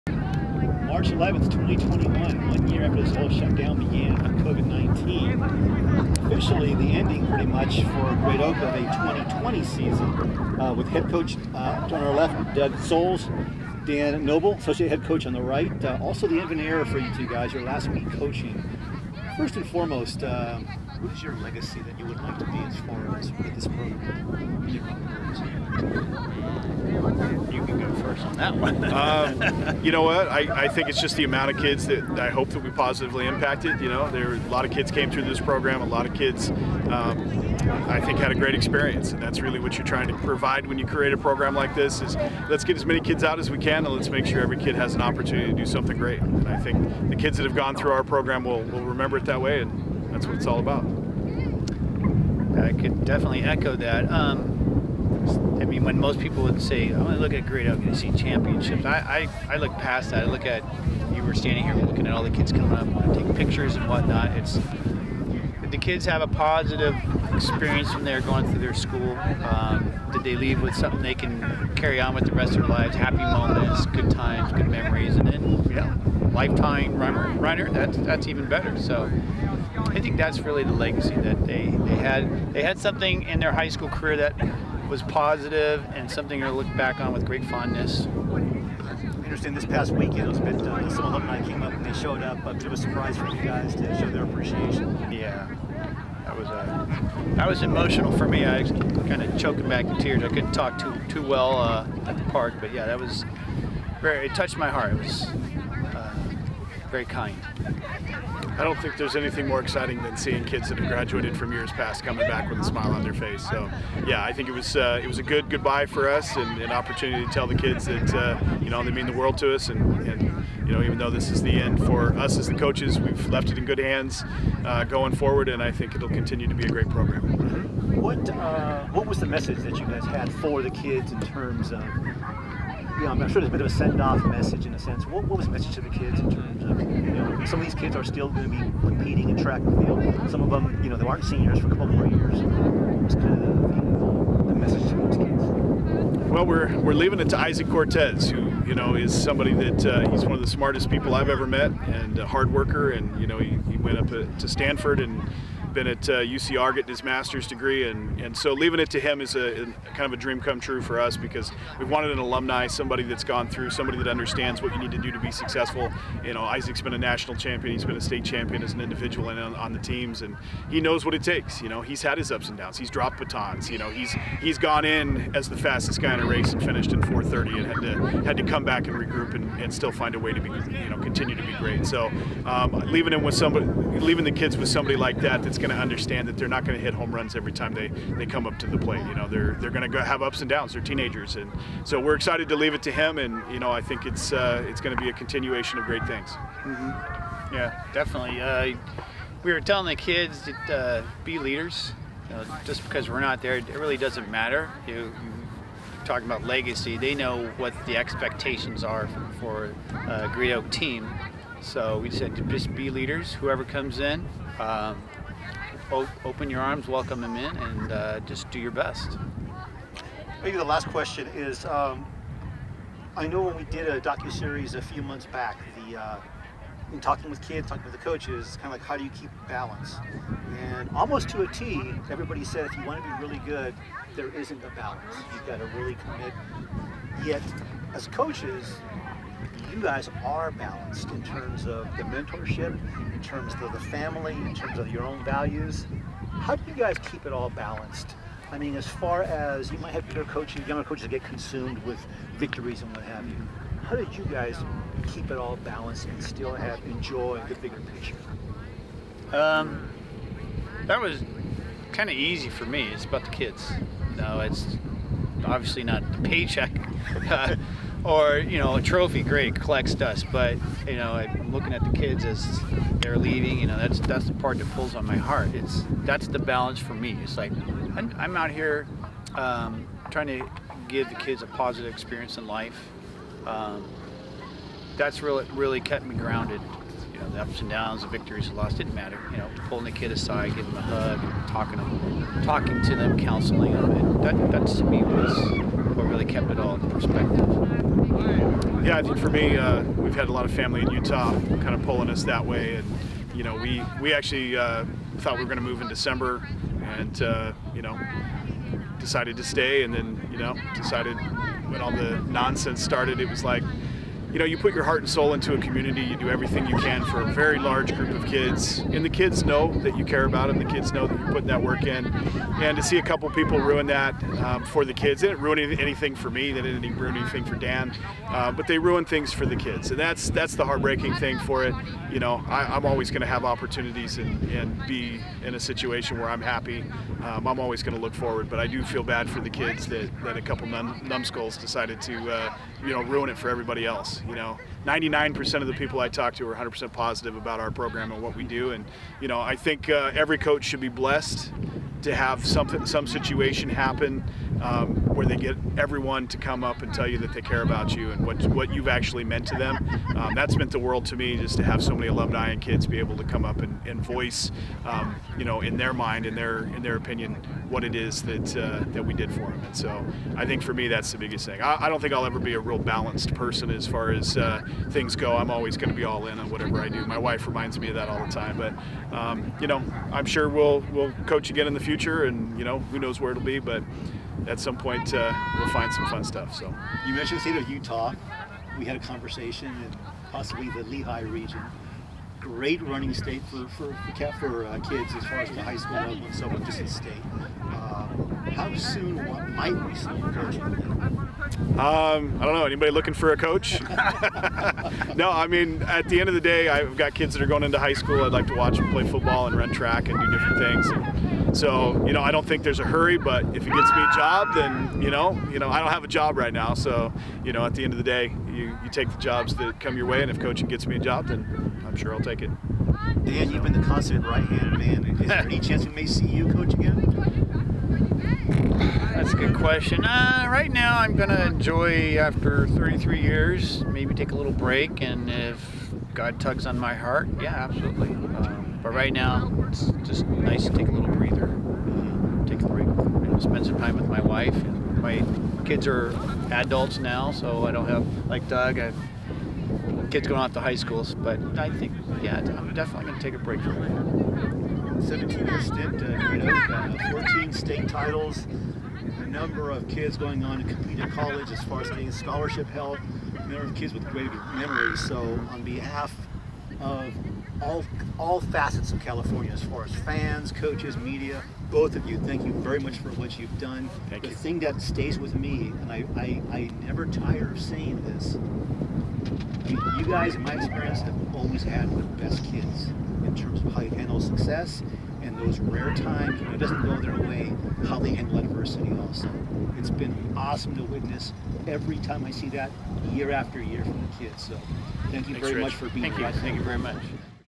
Great Oak's Doug Soles & Dan Noble in final coaching appearance at the school.
Over the course of 17 years, Great Oak head coach Doug Soles and associate head coach Dan Noble have created a dynasty in Temecula. Combining to orchestrate 14 state team titles in Division 1, the Wolfpack has developed into a national power. On March 11, 2021, the duo coached together for likely rhe final time. Doug will be moving to Utah while Dan is stepping back from the program. Here's their interview with Rich Gonzalez of PrepCalTrack.com after Great Oak swept the varsity and junior varsity team titles in both genders at the Southwestern League Finals.